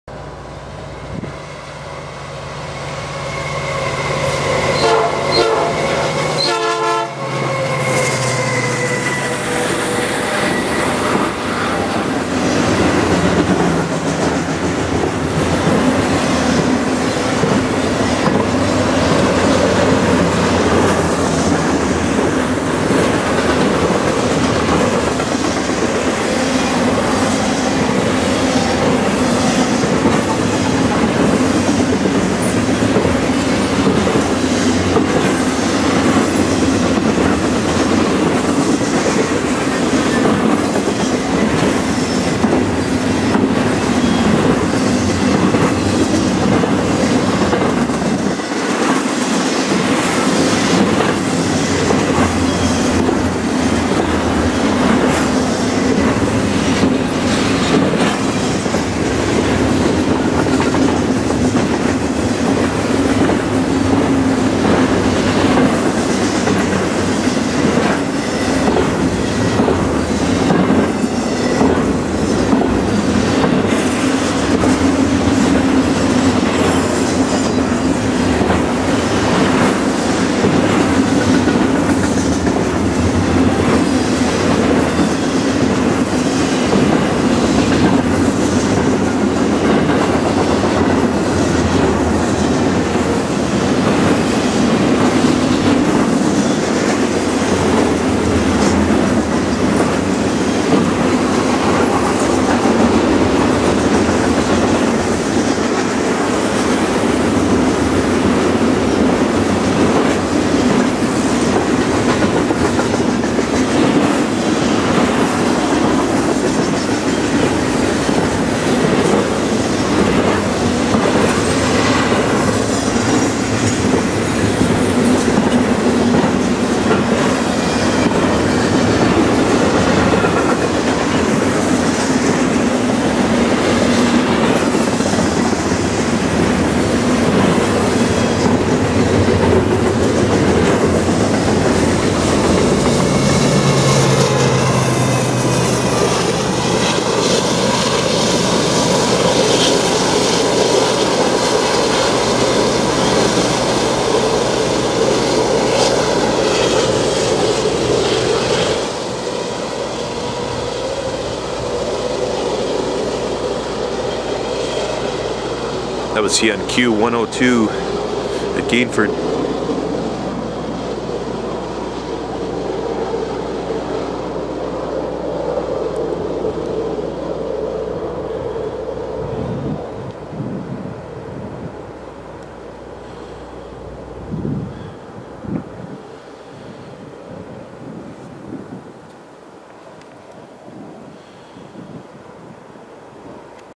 CN Train Spotting HD: CN 8912 & DPU 2234 Leads CN Q102 East w/ P5 Horn Show 14/21 12/9/16
Train ID: Q102
Axles: 436
Power: CN SD70M-2 8912 ES44DC 2234
Location: Gainford, AB
Milepost: N/A
Subdivision: Edson
Extra Notes: N/A